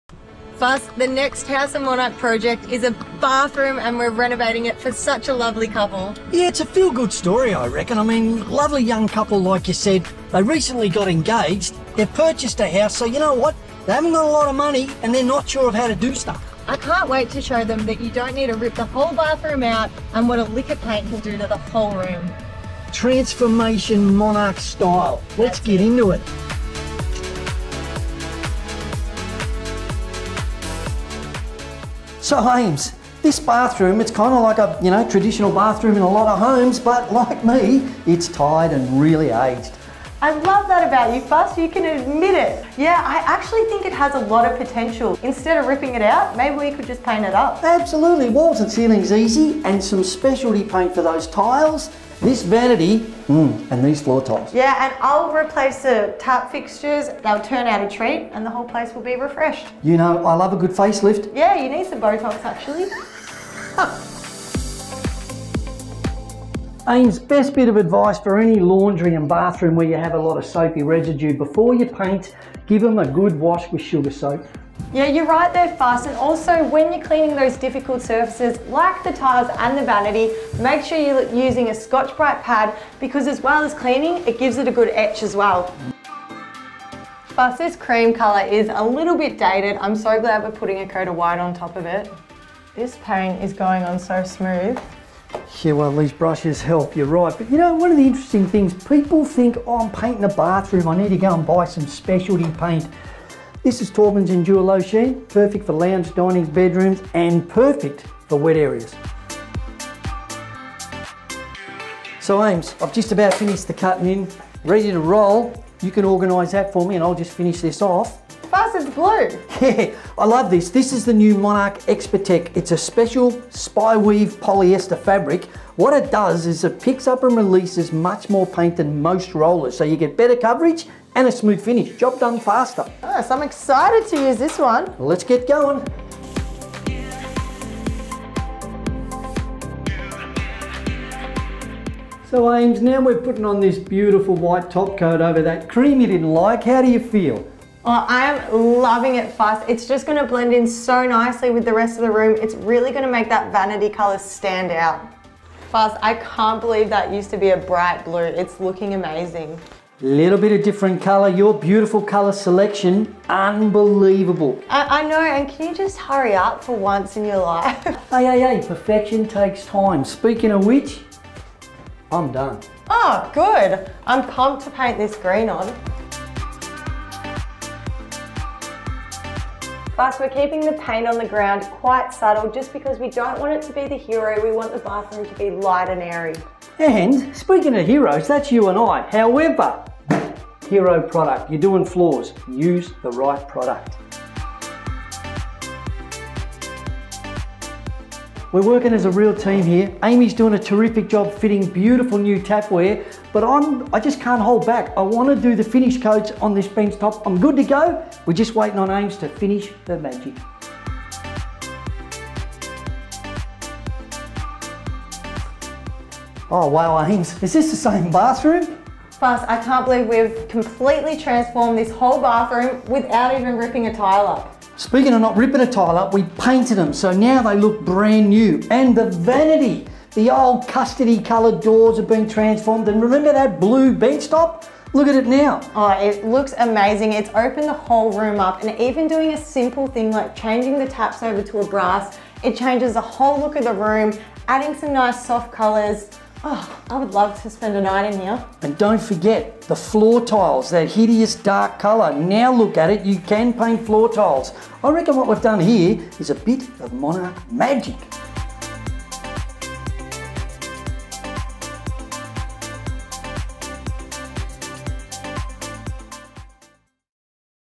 House of Monarch 2 - DIY Bathroom Makeover
In this House of Monarch episode, Mark and Aimee work some Monarch magic in this weeks House of Monarch transformation. The DIY duo show us how a fresh coat of paint and some upgraded accessories are all you need to breathe new life into a tired bathroom.

Learn more and find out where to buy on our